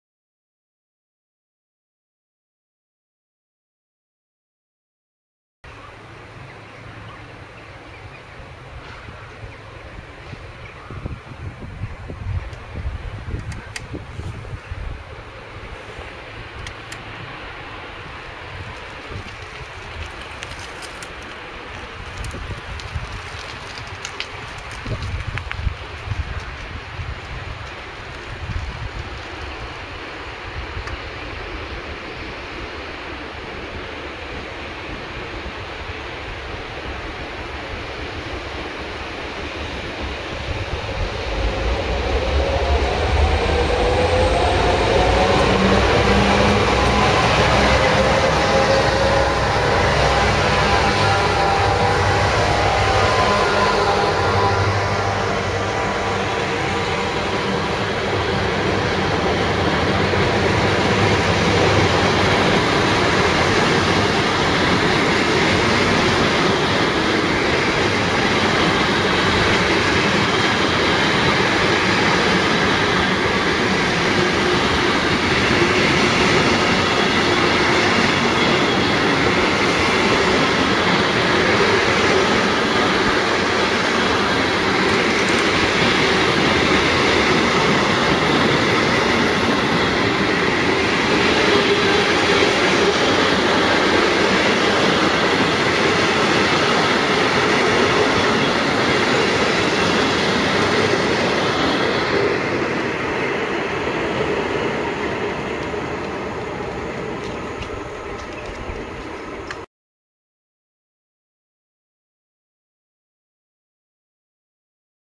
CFL 1023+1814+1805 med cementtog
CFL 1023+1814+1805 med cementtog passerer Kolding den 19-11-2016